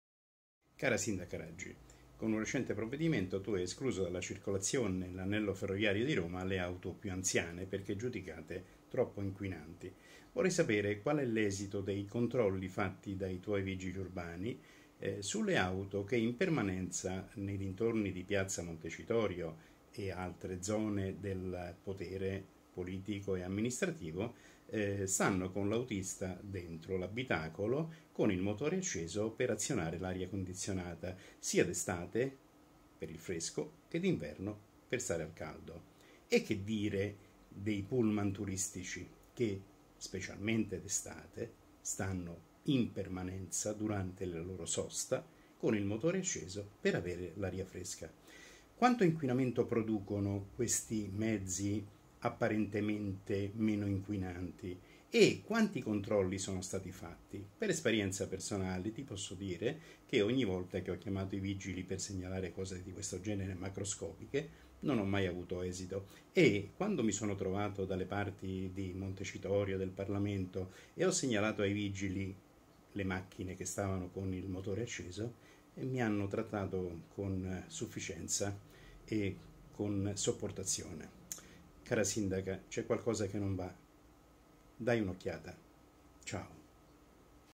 EURO DUE INQUINA, AUTOBLU E TORPEDONI ACCESI IN PERMANENZA?
Il punto del Direttore Editoriale di DISTAMPA Giacomo Carioti sulla lotta allo smog a Roma.